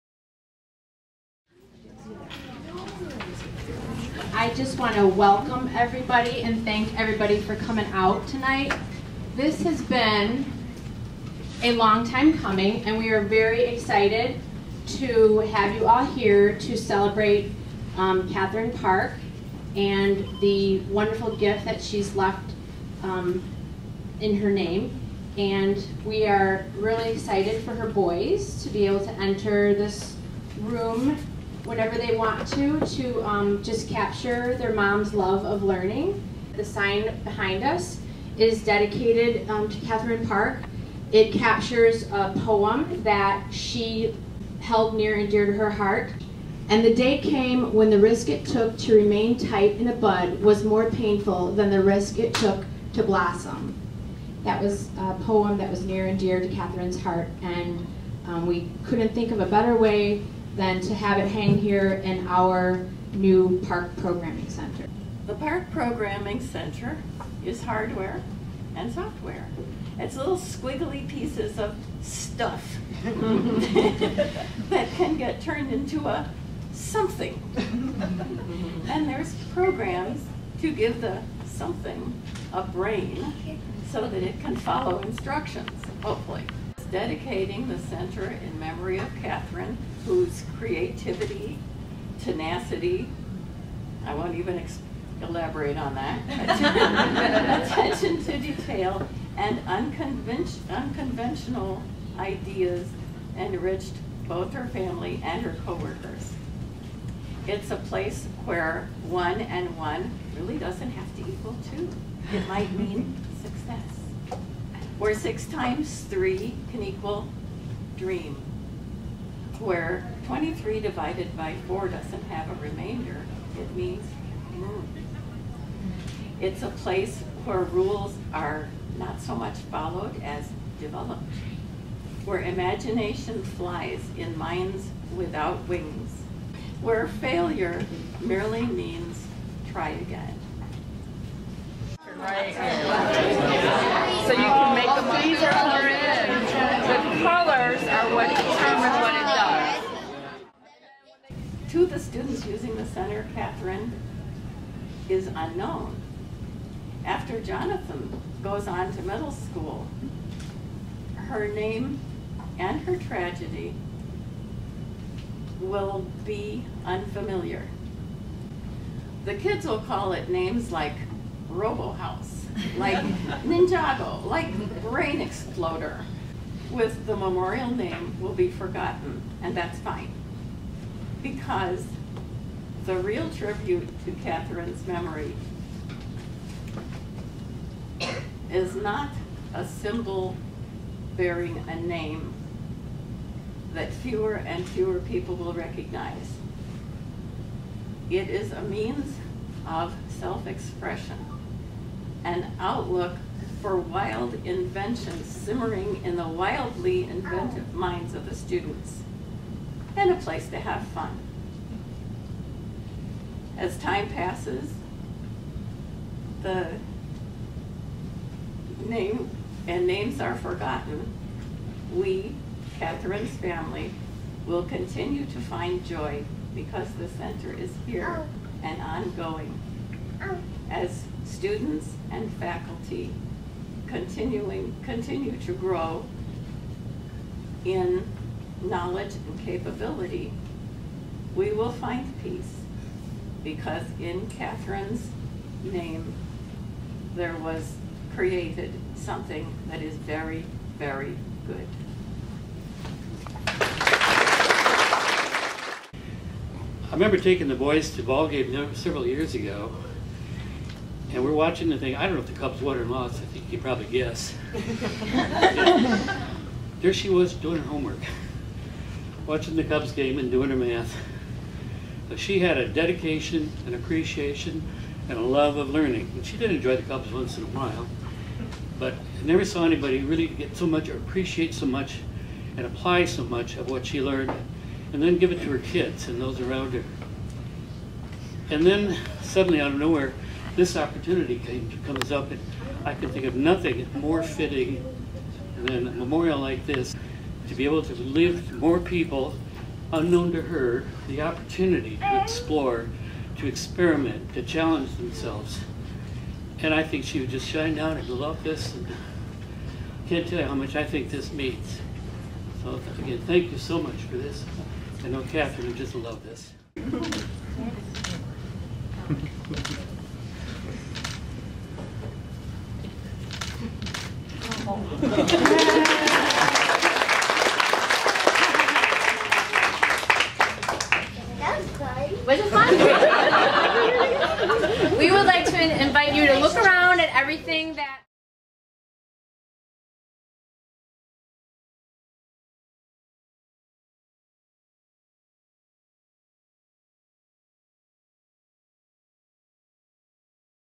Dedication of Park Programming Center at Westfield Elementary
On Thursday, August 31, Westfield Elementary School dedicated the new Park Programming Center, a space in the school’s library that will be used to teach students coding and programming skills.

The Programming Center is named in honor of Catherine Park, the mother of three CCSD 89 students. Catherine Park, 46, died unexpectedly during a family trip to Costa Rica in 2016. Her family made a $22,000 donation to provide opportunities for all students to learn more about the field she worked in.

Catherine Park attended Glen Crest Middle School, Glenbard South High School (where she was a National Merit finalist), and Bradley University. At the time of her death, she was working as a Senior Clearing Technology Developer for Getco.

The school has purchased robots and other computer programming tools that will be accessible to students from kindergarten through fifth grade. Catherine’s mother, Anna Byrne, spoke at the dedication ceremony. She said the family is thrilled to know that Catherine’s “creativity, tenacity, attention to detail, and unconventional ideas” will live on in the Center.

Byrne hopes the Center will be a place were rules “are not so much followed as developed, where imagination flies in minds without wings, where failure merely means try again.”

“The real tribute to Catherine’s memory is not a symbol bearing a name that fewer and fewer people will recognize,” Byrne said. “It is a means of self-expression, an outlook for wild inventions simmering in the wildly inventive minds of the students - and a place to have fun. As time passes, and names are forgotten, Catherine’s family will continue to find joy because the Center is here. As students and faculty continue to grow in knowledge and capability, we will find peace.”

Kenneth Park met his future wife on a mission trip to China in 2000. He grew up in South Korea, in a home with no electricity or refrigerator. But, he had a junior high teacher that changed his life. Kenneth believes Westfield will use the technology to challenge students and give them the opportunities that a teacher once gave him.

“The passion that she (Catherine) had, I would like that be to transmitted to young people,” Kenneth Park said. “I hope this program will become contagious in a positive way to other schools.”